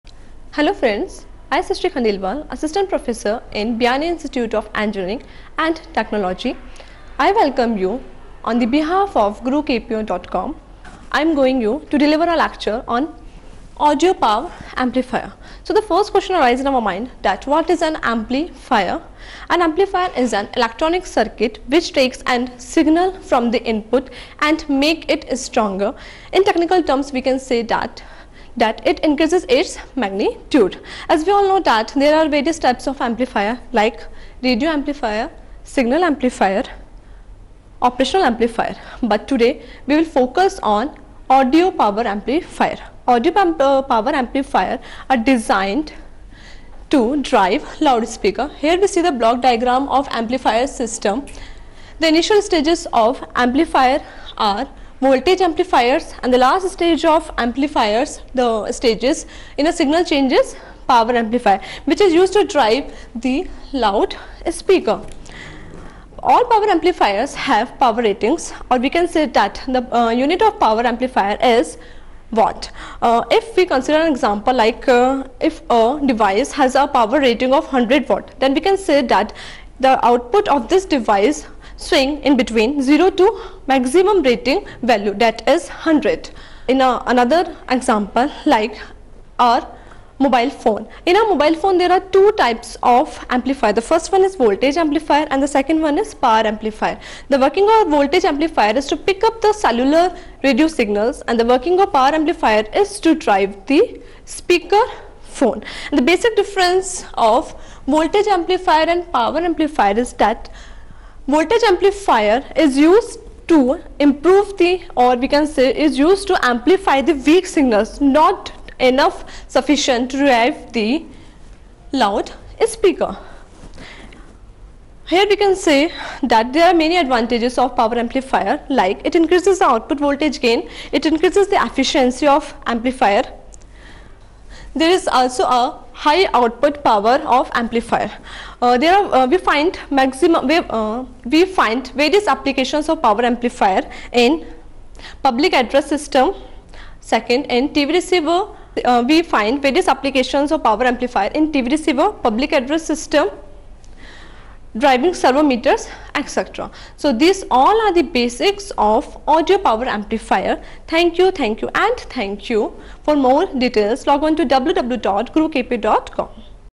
Audio Power Amplifier by Ms. Shristy Khandelwal, Biyani Girls College, Jaipur, Rajasthan 2014
In this video Ms. Shristy Khandelwal, asst. professor, Biyani Girls College, explains the purpose of power amplifier which takes a signal from a source device and makes it suitable for driving a loud speaker. Audio power amplifiers are those amplifiers which are designed to drive loud speakers .Power is not really something that can be "amplified". Voltage and current can be amplified.